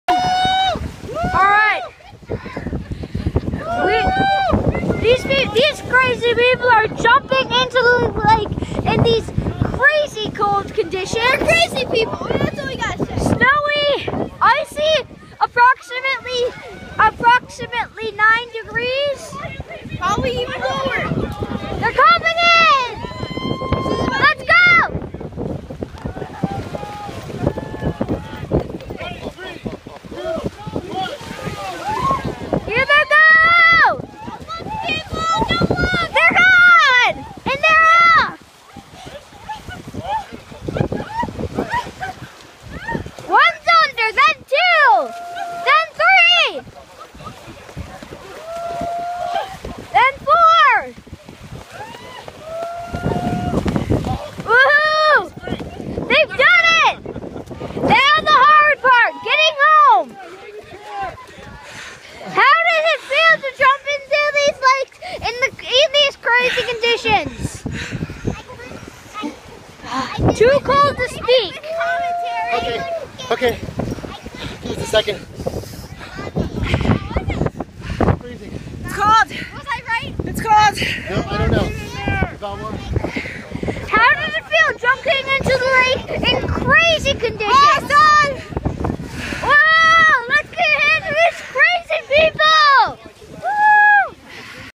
2014 Polar Bear Plunge Klode Park Beach
5th Annual New Year's Day plunge into Lake Michigan via Klode Park Beach, Whitefish Bay, Wisconsin.